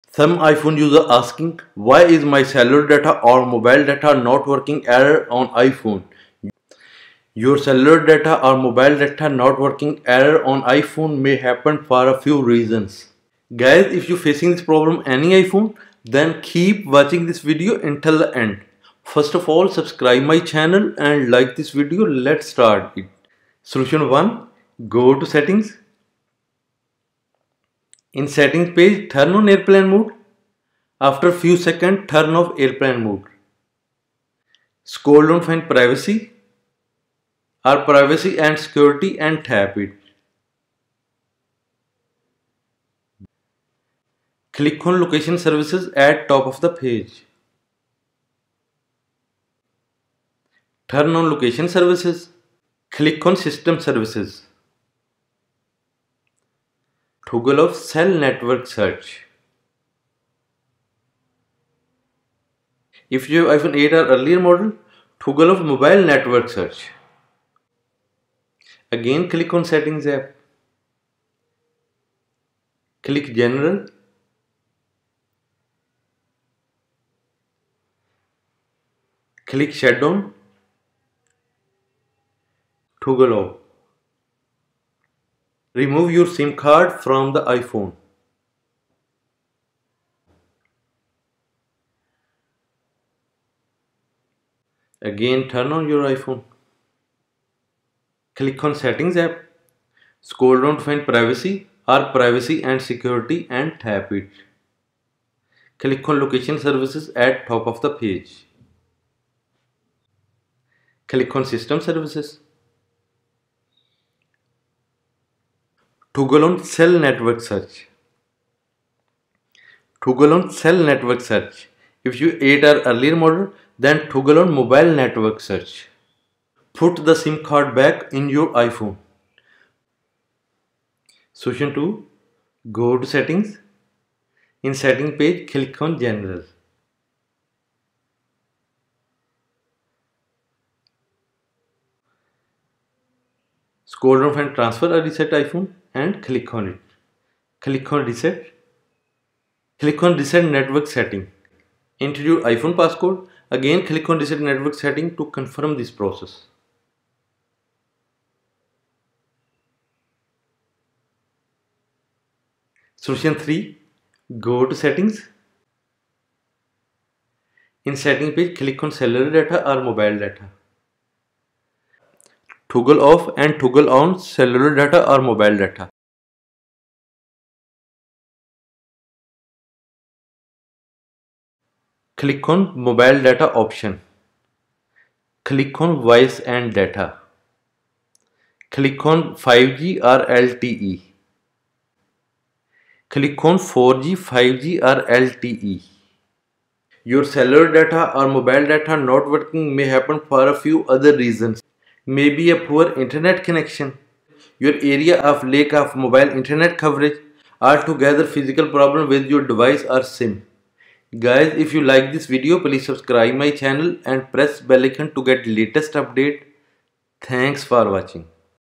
How To Fix ‏Mobile Service/Mobil DataNot Working on iPhone,Why my iphone Cellular,stuck Searching
this video about Mobile Service Not Working on iPhone  ios18,
why is my cellular data or i mobile date not working on iPhone?
iphone on searching?